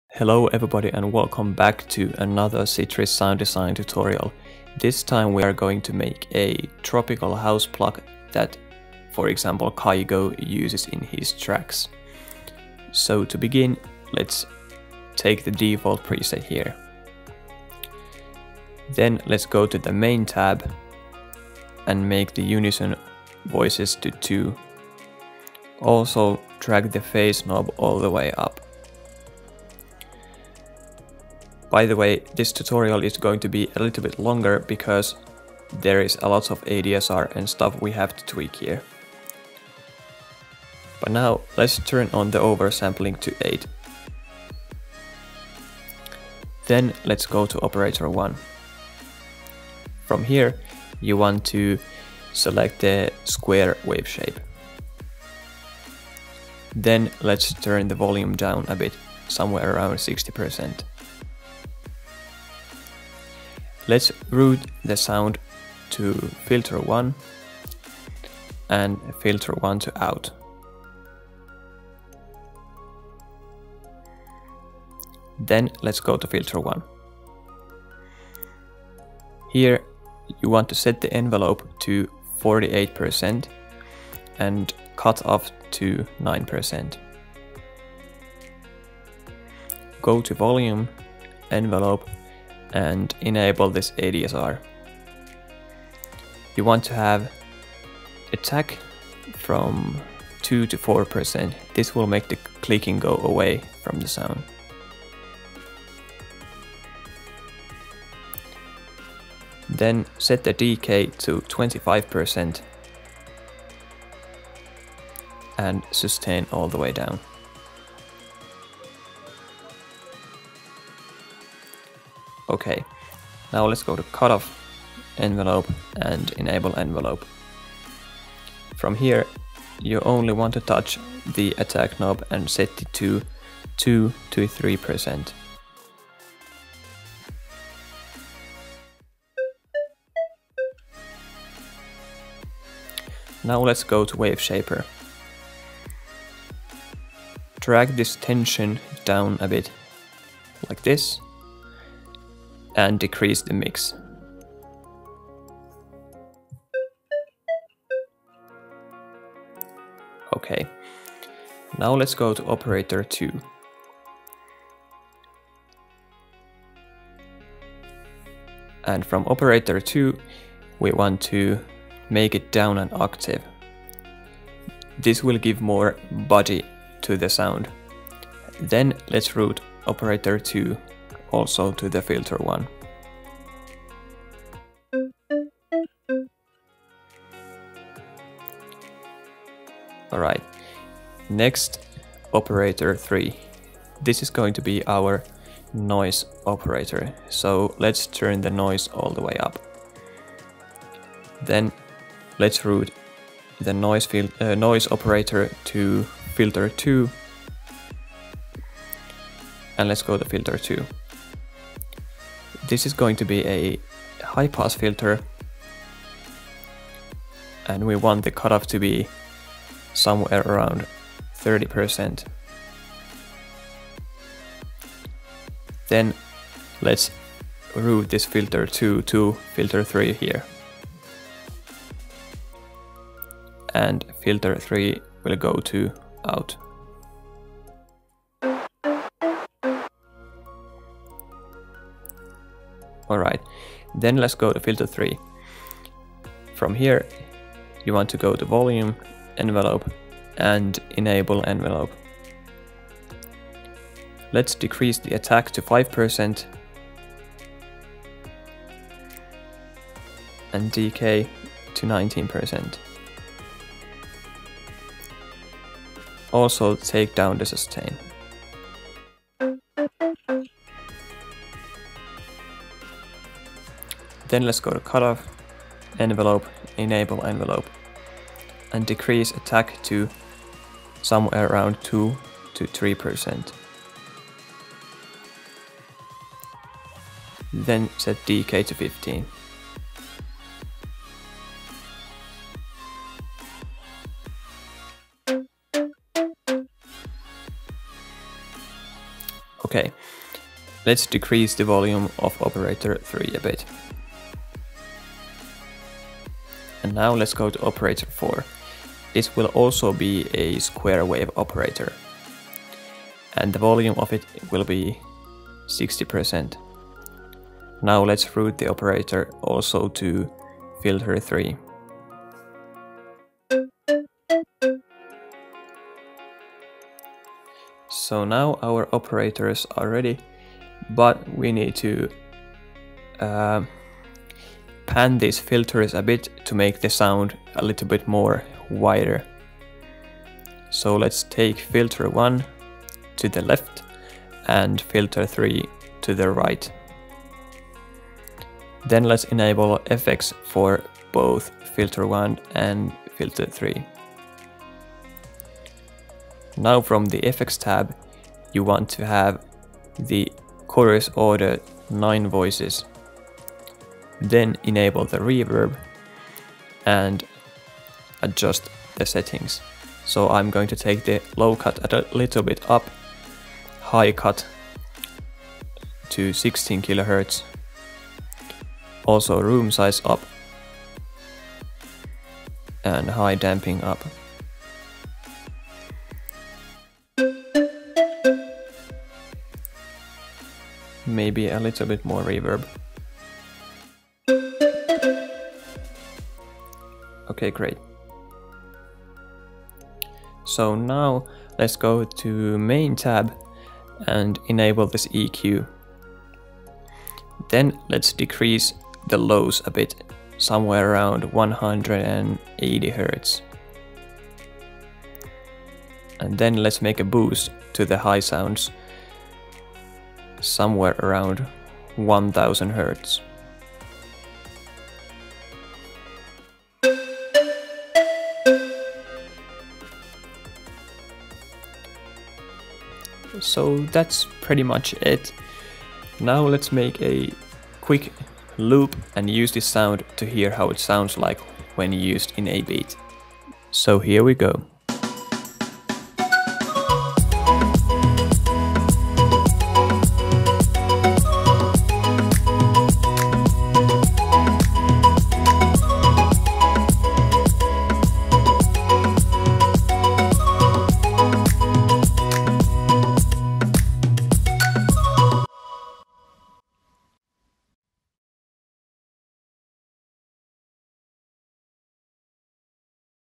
Sound Design with Sytrus: Tropical House Pluck (Kygo, Matoma, etc.)
Make sure to watch until the end to hear how it sounds like when used with other instruments :)
PS. Here's a drinking game for you: Take a shot every time I say "Let's"